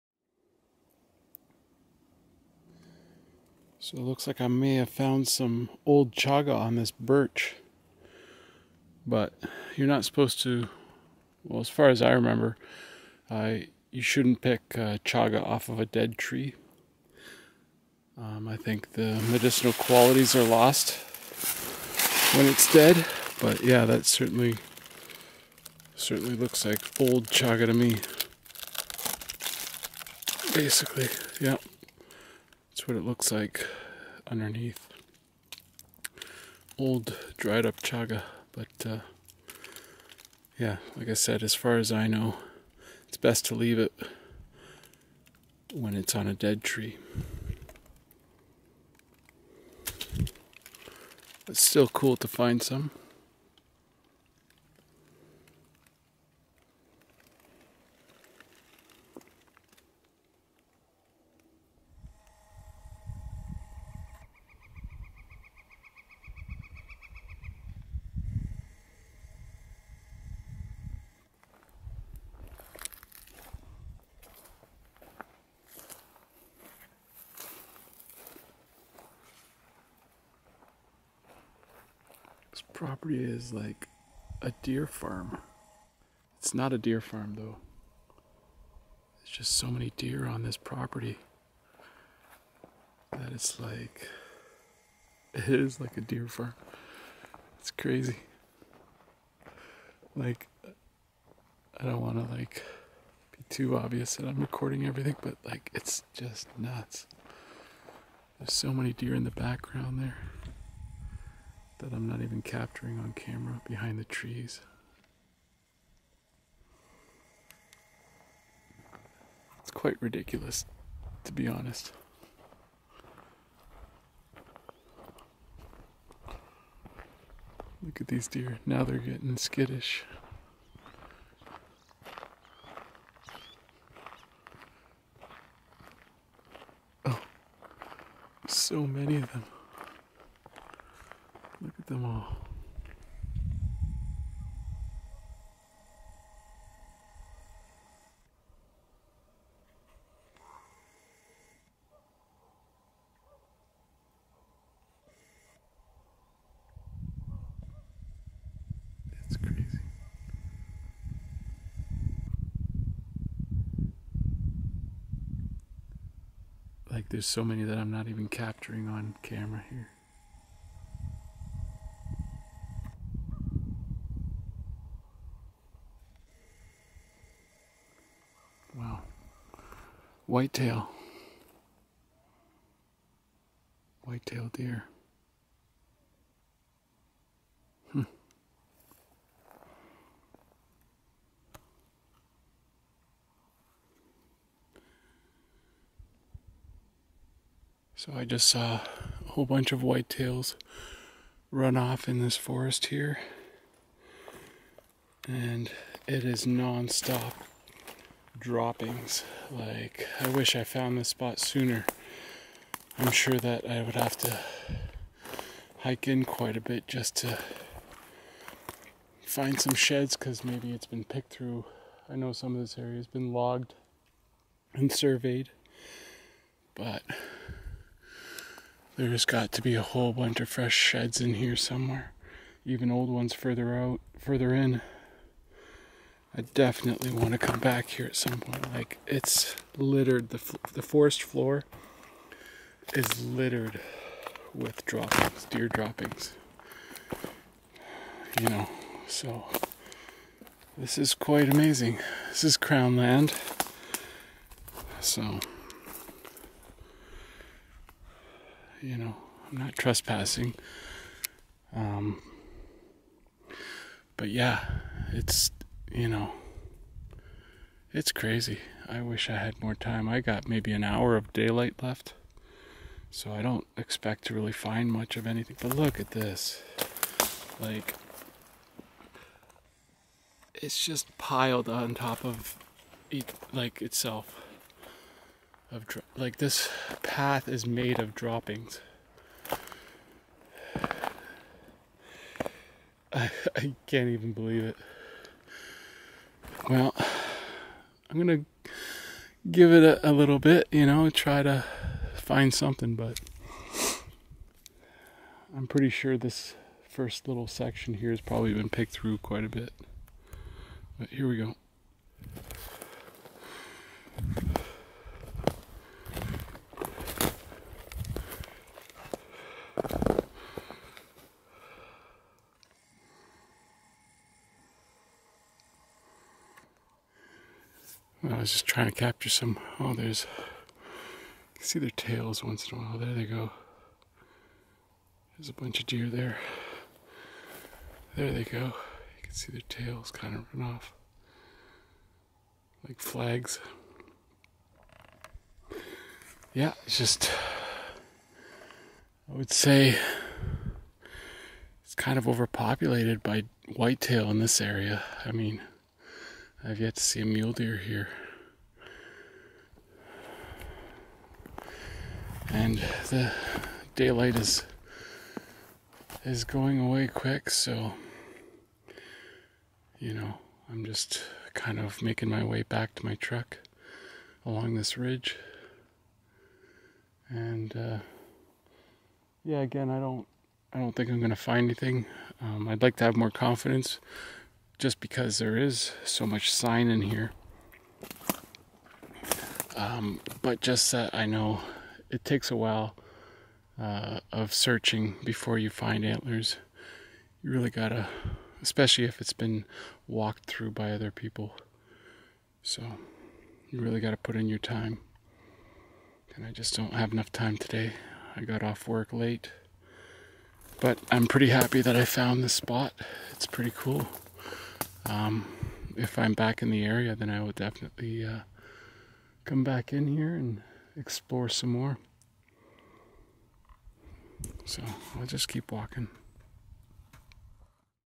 Huge population of White-tailed Deer Found in BC, 2022
Filmed on April 13, 2022 in British Columbia, Canada.